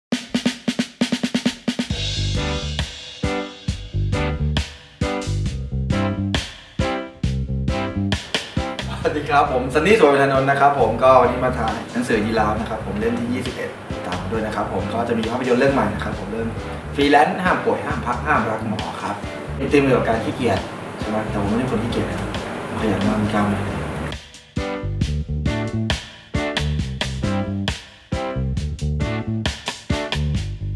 giraffe issue 21 - SLEEPING LAZY [Sunny Suwanmethanont]
giraffe issue 21
LAZY ISSUE

ยีราฟฉบับนี้ชวนเปิดประเด็นประวัติศาสตร์ของ 'ความขี้เกียจ' เปิดใจมองอีกด้านของการอยู่เฉยๆ ในสังคมที่เชิดชูการทำงานหนัก และมอบรางวัลติดป้ายดีเด่นให้แก่คนทุ่มเทเพื่อหน้าที่การงาน
พร้อมบทสัมภาษณ์ 'ซันนี่ สุวรรณเมธานนท์' หนุ่มมาดเซอร์ผู้หลงรักเพื่อนสนิท สายลับผู้คอยไล่จับเมียน้อย หรือฟรีแลนซ์ผู้หมกมุ่นกับการทำงานในหนังเรื่องล่าสุด ที่ว่ามานี้คือตัวอย่างบทบาทอันหลากหลายที่เขาได้รับตลอดสิบปีที่ผ่านมา แต่บทบาทที่เขารับมาอย่างยาวนานยิ่งกว่าในชีวิตจริงคือการเป็นนักแสดง ...ทำไมเข้าถึงไม่เบื่อ ไม่พัก และยังรักสิ่งนี้อย่างหัวปักหัวปำ—คำตอบที่เขาตอบมาในบทสัมภาษณ์นั้นช่างน่าอิจฉา!

จันทร์ที่ 7 กันยายนนี้
ดูจุดแจก/จุดวาง และอ่านแบบออนไลน์ได้ที่

no shy, grab one, it's FREE!
1/2 SEPTEMBER 2015